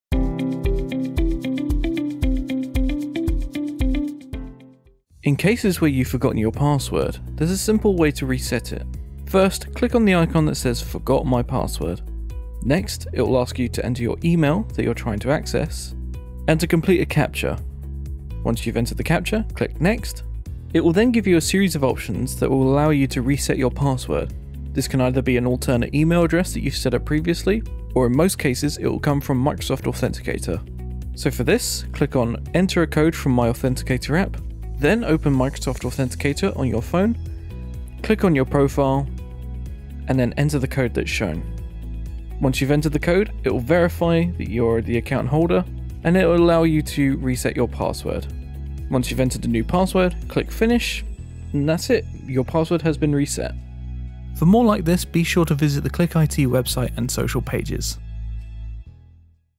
Self Service Password Reset Microsoft 365
This step-by-step video will guide you through the process of setting up and using the Self Service Password Reset feature in Microsoft 365. You'll learn how to securely reset your password, ensuring you have continuous access to your account.